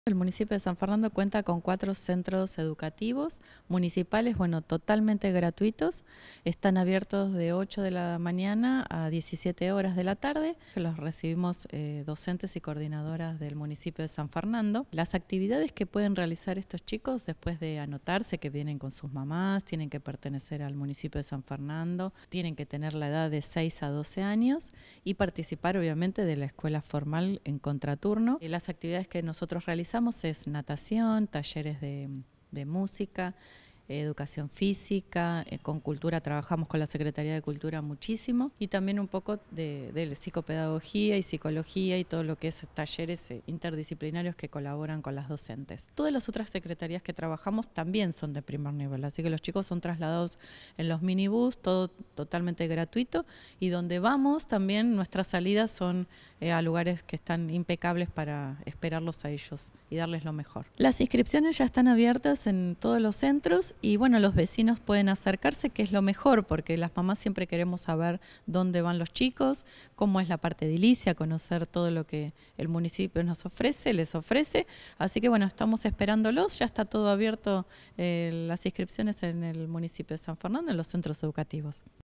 Los Centros Educativos Integrales abrieron las inscripciones 2019
El Municipio informó que se encuentran abiertas las inscripciones de los cuatro Centros Educativos Integrales para chicos de 6 a 12 años.